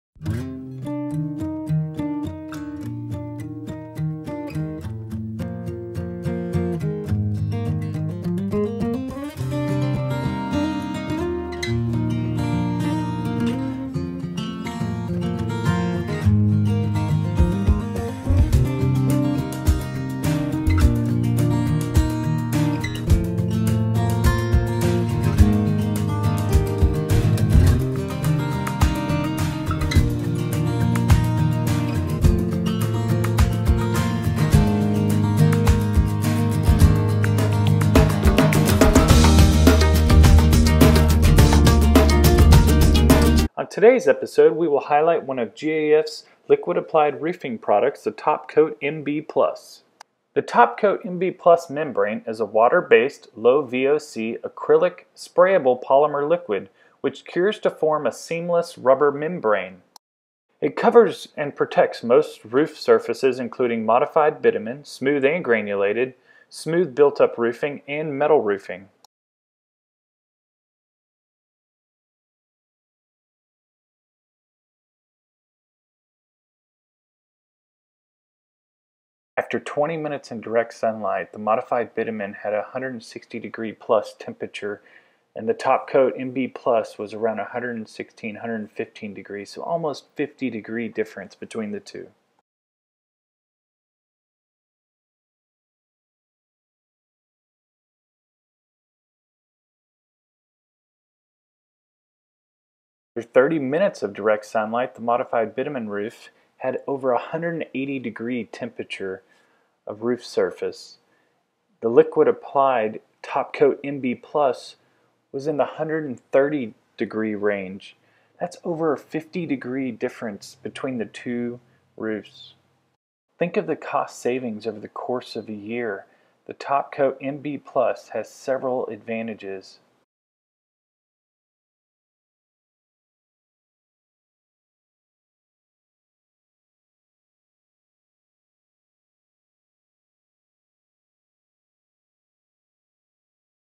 GAF TopCoat vs Modified Bitumen
GAF TopCoat MB Plus liquid applied roofing system is put to the test. Using a temperature gun we conducted two different test comparing a modified bitumen roof versus the GAF coated modified bitumen. The results are crazy...